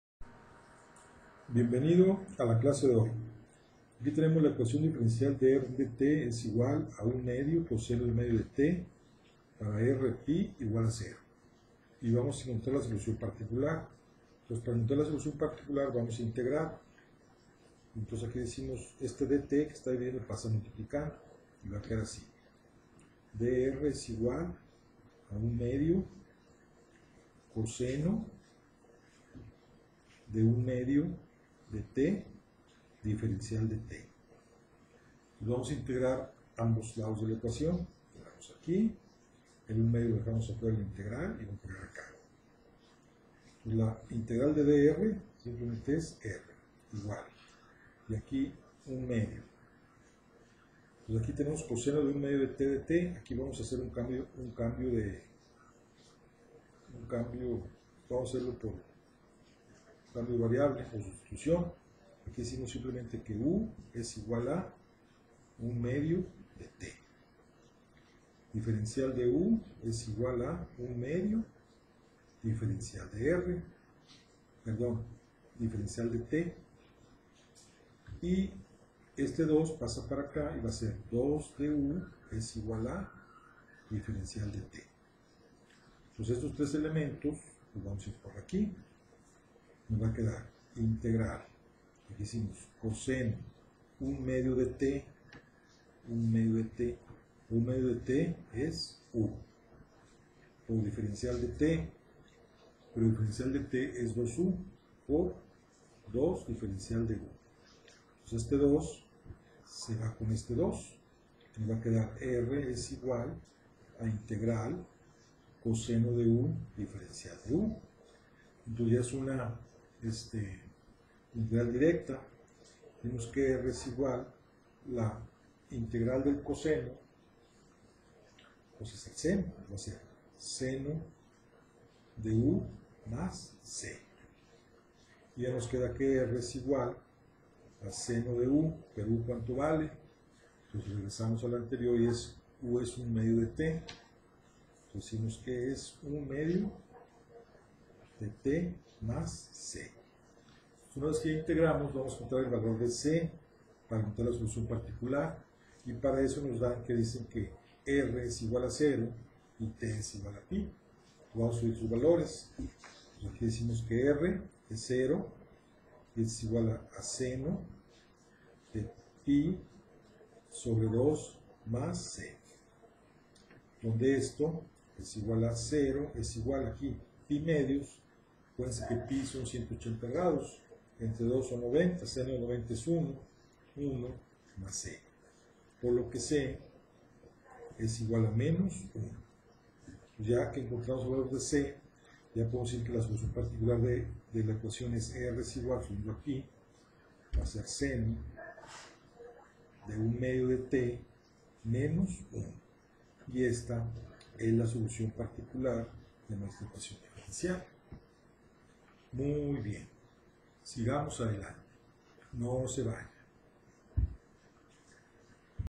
Ecuaciones diferenciales 57
Ecuaciones diferenciales. Metodo de separación de variables, explicadas paso a paso, fáciles de entender, se recomienda ver del 1 er video al ultimo de estas clases magistrales.